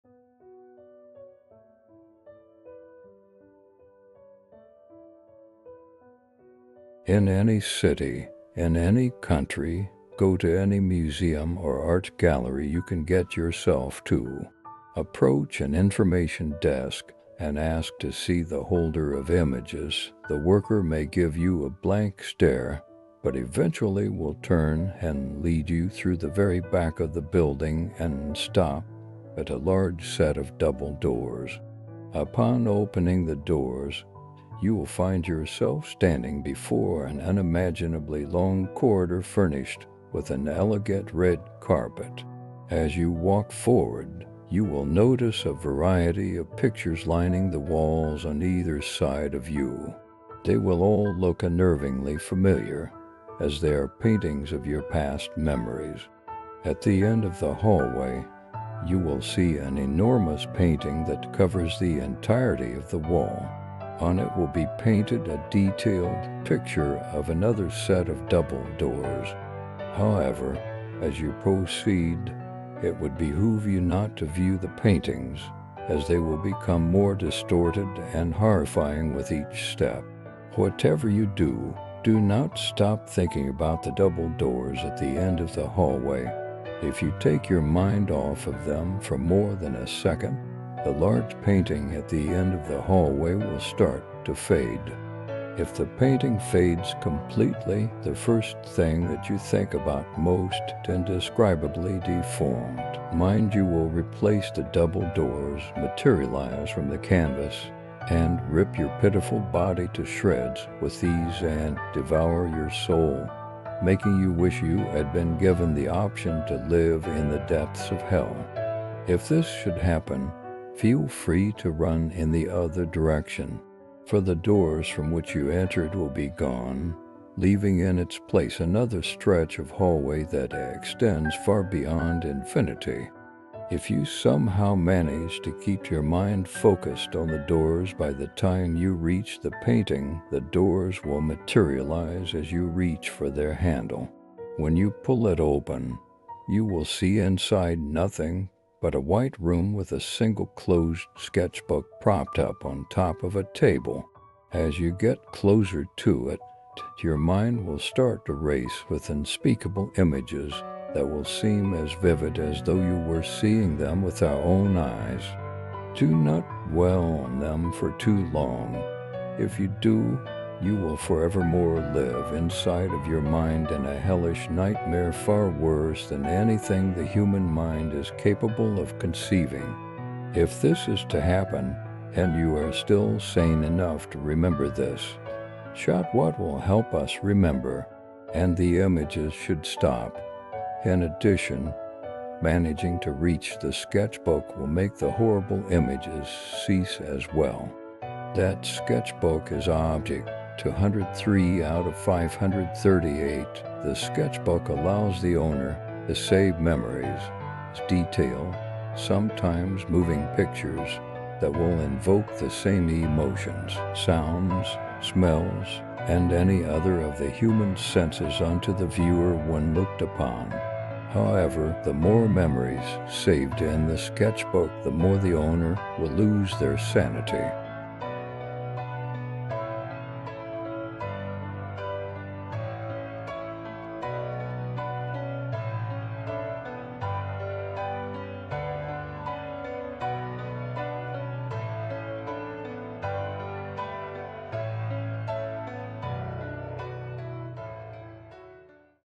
The Holder Series | Holder of Images
Posted by: Scooter4849

Written by: Nathan Kelly

By: @Myuu

00:00 Holder of Images
03:18 Object 203 of 538
03:49 End Card